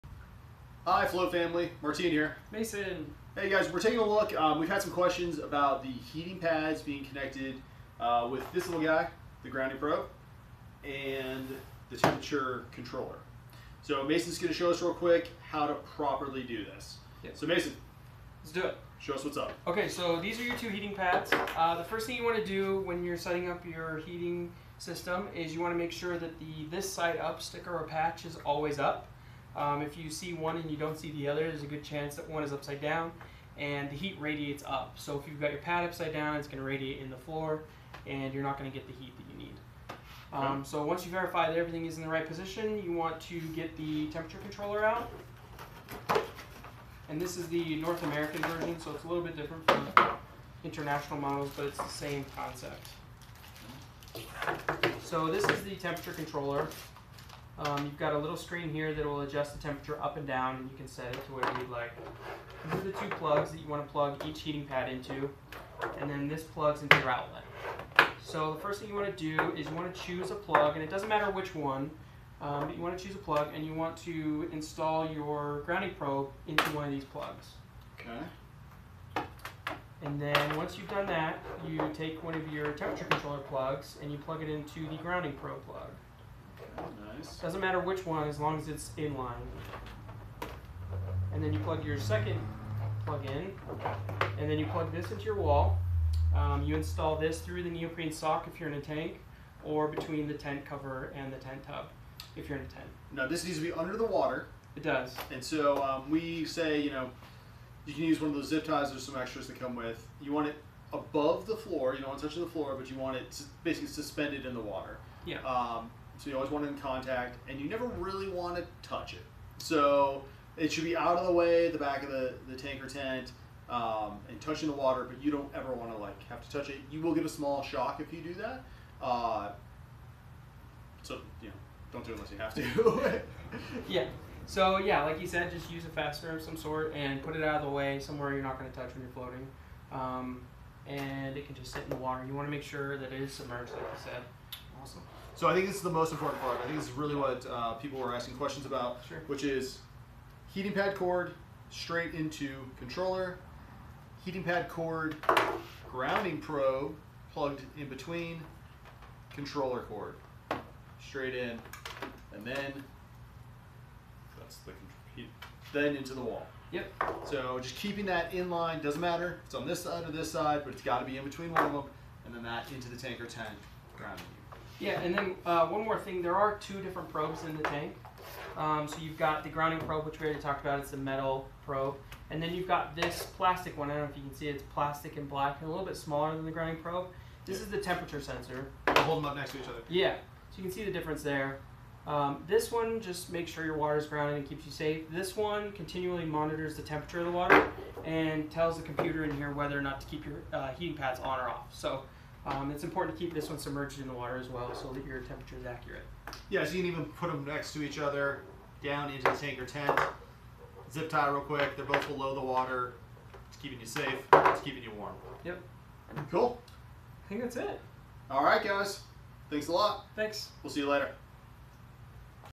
Plugging In the Heating Pads
Here is a quick how-to on plugging the heating pads for your Zen Float Co Tank or Tent together.